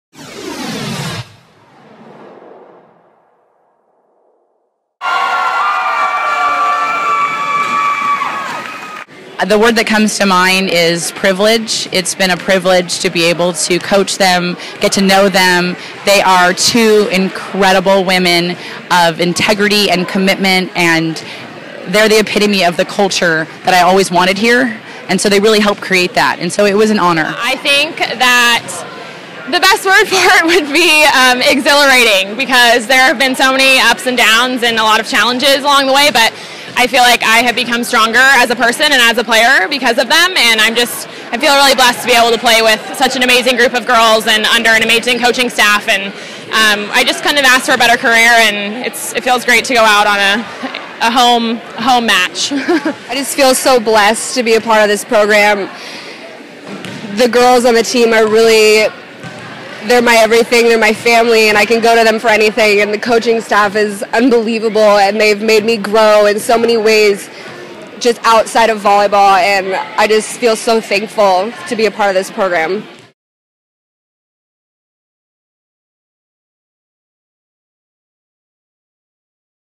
VB SN
Interview with Volleyball Head coach Jody Garry and Senior players Kelsey Johnston and Julia Ashen after the senior night loss to UCSD.